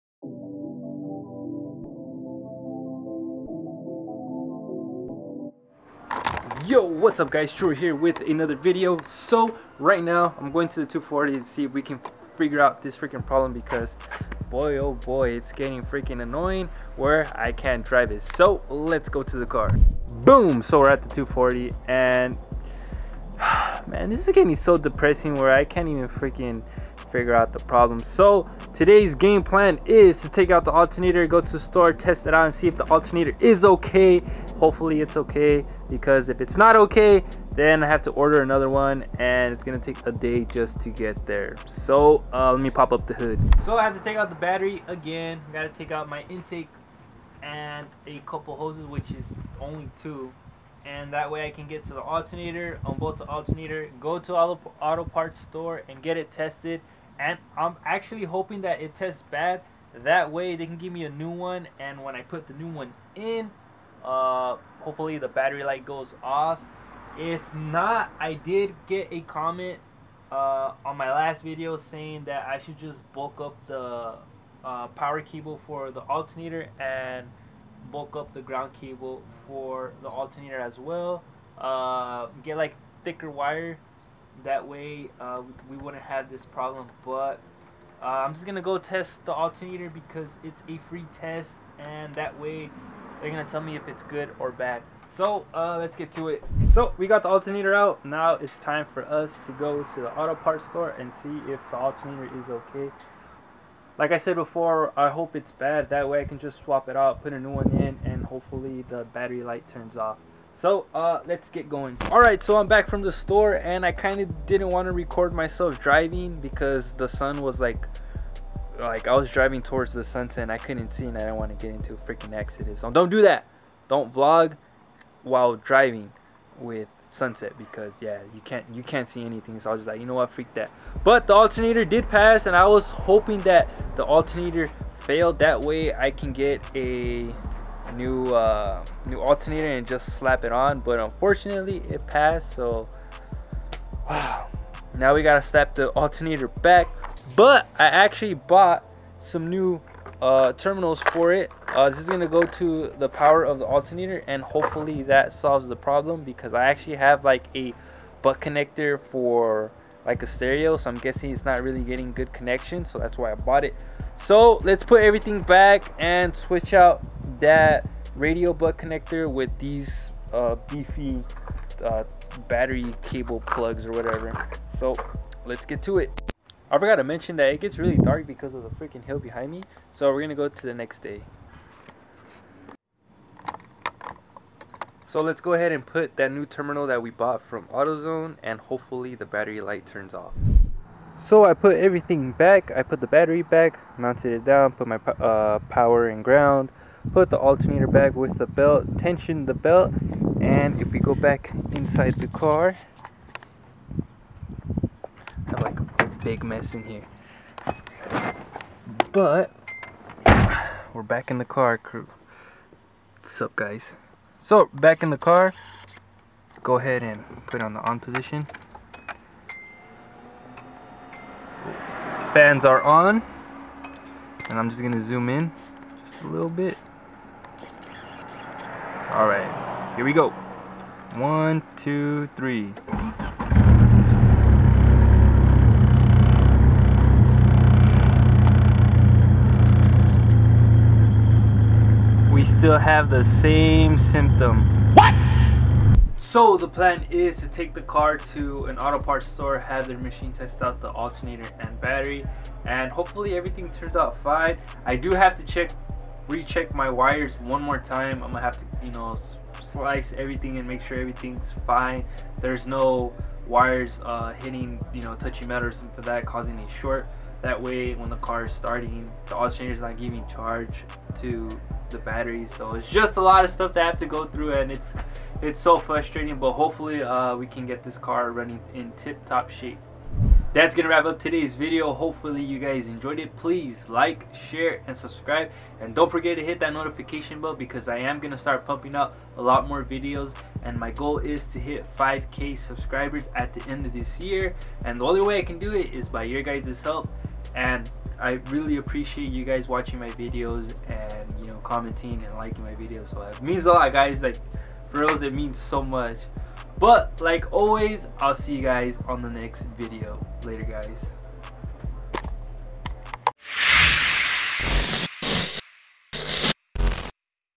Trying To Solve The Issue
I am stilling trying to figure out the problem. Hopefully the next parts I get will finally fix my charging issues. Not driving the 240sx is really depressing.

Let's try and blast 10 likes in this video.